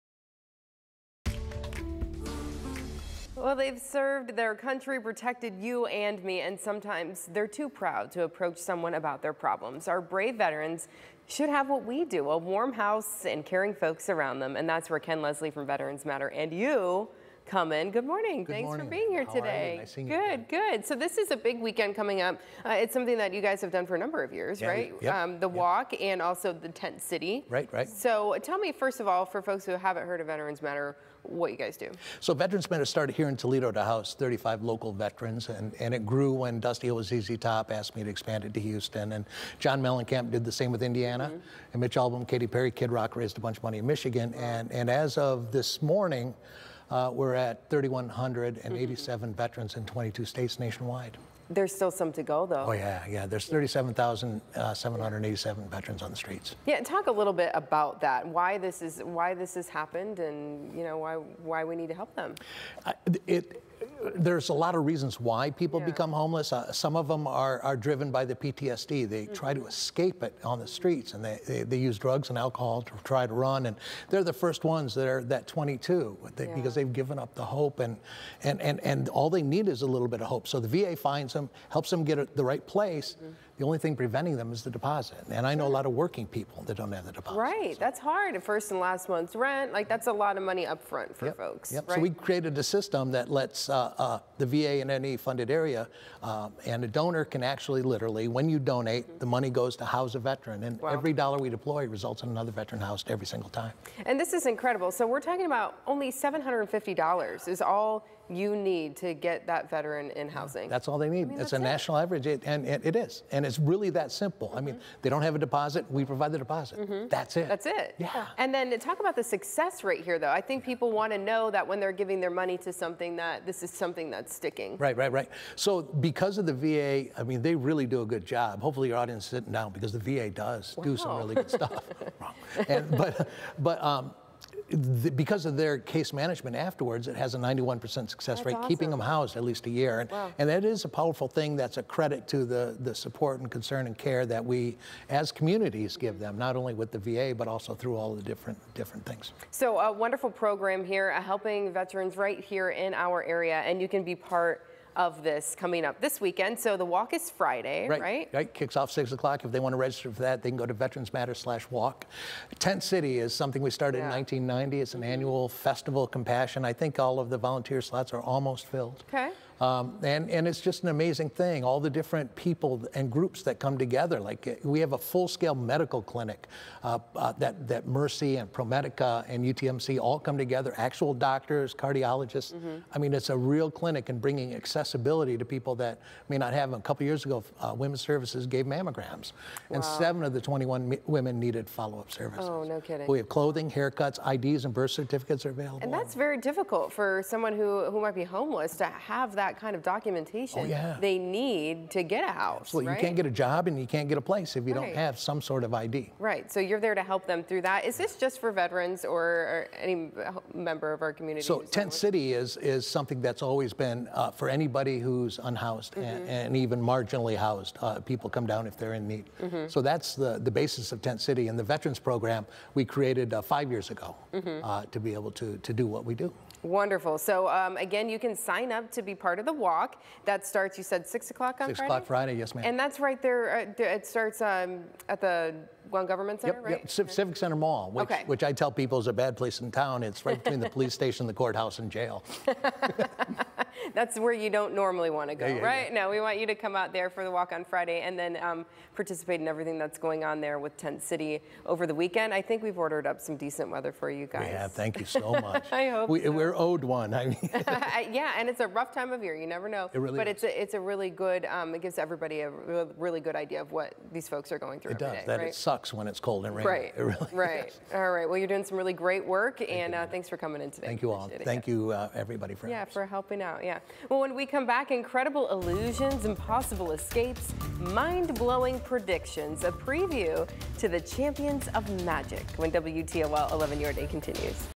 Walk to End Veteran Homelessness takes place Friday
Walk begins at the Civic Center Mall behind the Safety Building at 6 p.m. Vets/active duty admission is $20, general public $25, students $15.